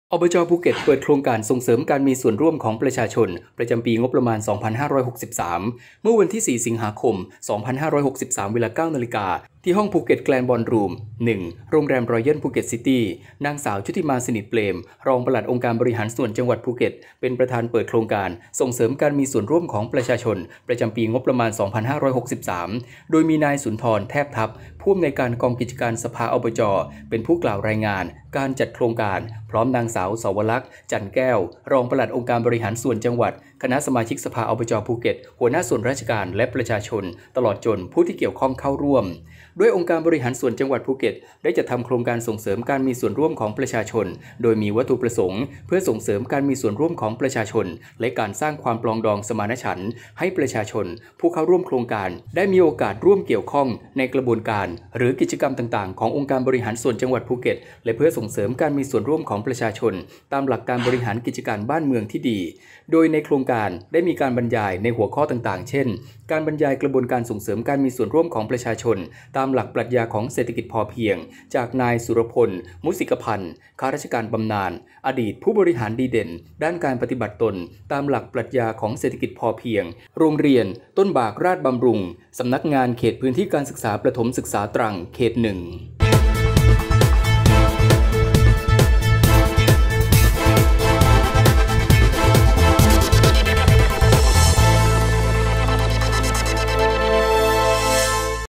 อบจ.ภูเก็ต เปิดโครงการส่งเสริมการมีส่วนร่วมของประชาชน ประจำปีงบประมาณ 2563
เมื่อวันที่ 04 สิงหาคม 2563 เวลา 09.00 น. ที่ห้องภูเก็ตแกรนด์บอลรูม 1 โรงแรมรอยัลภูเก็ตซิตี้ นางสาวชุติมา สนิมเปรม รองปลัดองค์การบริหารส่วนจังหวัดภูเก็ต เป็นประธานเปิดโครงการส่งเสริมการมีส่วนร่วมของประชาชน ประจำปีงบประมาณ 2563 โดยมีนายสุนทร แทบทับ ผู้อำนวยกองกิจการสภา อบจ. เป็นผู้กล่าวรายงานการจัดโครงการฯ พร้อมนางสาวเสาวลักษณ์ จันทร์แก้ว รองปลัดองค์การบริหารส่วนจังหวัด คณะสมาชิกสภา อบจ.ภูเก็ต หัวหน้าส่วนราชการ และประชาชน ตลอดจนผู้เกี่ยวข้อง เข้าร่วม

ด้วยองค์การบริหารส่วนจังหวัดภูเก็ต ได้จัดทำโครงการส่งเสริมการมีส่วนร่วมของประชาชน โดยมีวัตถุประสงค์เพื่อส่งเสริมการมีส่วนร่วมของประชาชน และการสร้างความปรองดองสมานฉันท์ ให้ประชาชนผู้เข้าร่วมโครงการได้มีโอกาสร่วมเกี่ยวข้องในกระบวนการหรือกิจกรรมต่าง ๆ ขององค์การบริหารส่วนจังหวัดภูเก็ต และเพื่อส่งเสริมการมีส่วนร่วมของประชาชนตามหลักการบริหารกิจการบ้านเมืองที่ดี โดยในโครงการฯ ได้มีการบรรยายในหัวข้อต่าง ๆ เช่น การบรรยายกระบวนการส่งเสริมการมีส่วนร่วมของประชาชนตามหลักปรัชญาของเศรษฐกิจพอเพียง จากนายสุรพล มุสิกพันธ์ ข้าราชการบำนาญ อดีตผู้บริหารดีเด่นด้านการปฏิบัติตนตามหลักปรัชญาของเศรษฐกิจพอเพียง โรงเรียนต้นบากราฎร์บำรุง สำนักงานเขตพื้นที่การศึกษาประถมศึกษาตรัง เขต 1